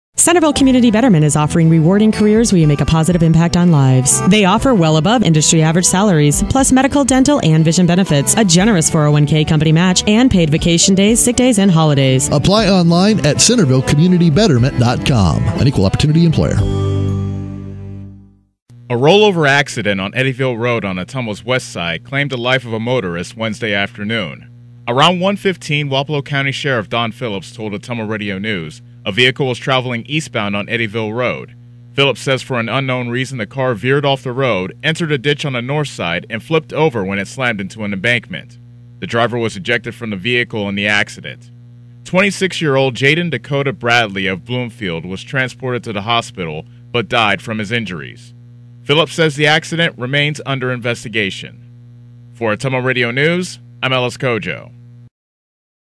SE Iowa Man Dies in Single Car Accident
A Wednesday afternoon rollover accident claimed the life of a Bloomfield man.